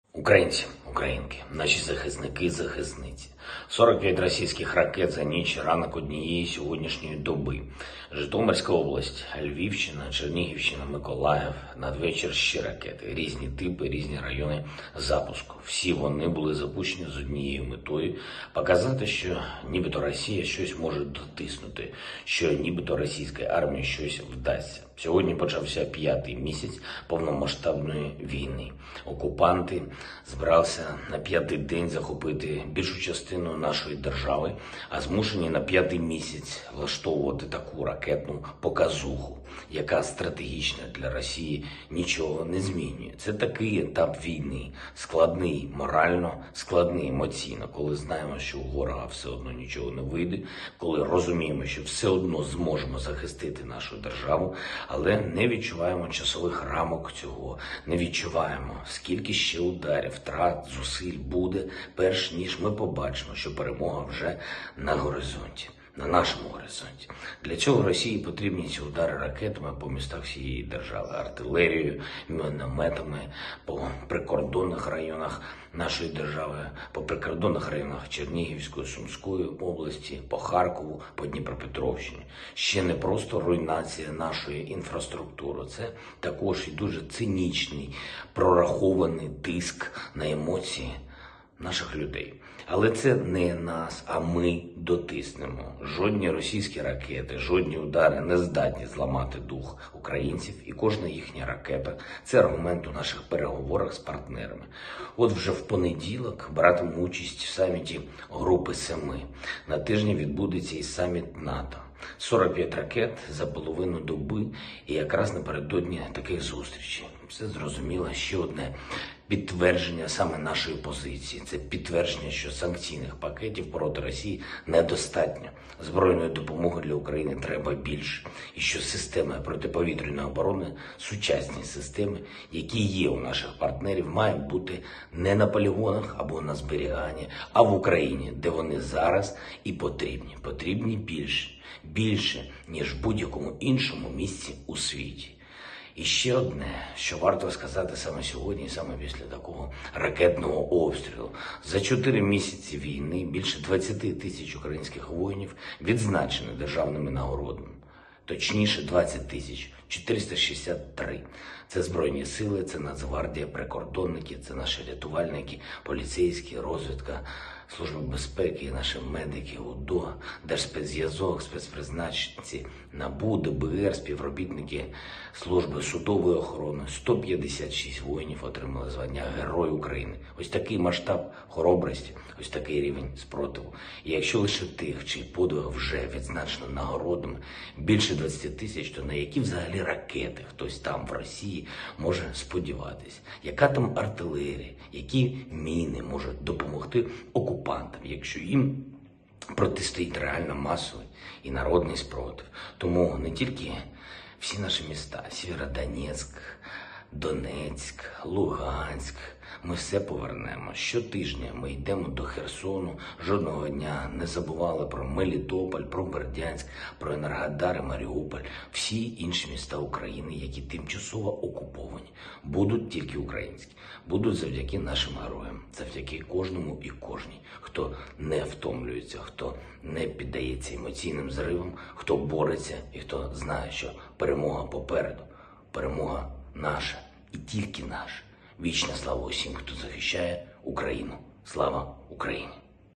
Address by Volodymyr Zelensky at the end of the 122-nd day of the war
Today marks the fifth month of a full-scale war. The occupiers were going to capture most of our country on the fifth day, but have to stage such a missile show on the fifth month, which does not change anything strategically for Russia. This is such a phase of the war - morally difficult, emotionally difficult. When we know that the enemy will not succeed, when we understand that we will still be able to defend our state, but we do not feel the time limits of this, we do not feel how many more strikes, losses, efforts will be before we see that victory is already on our horizon.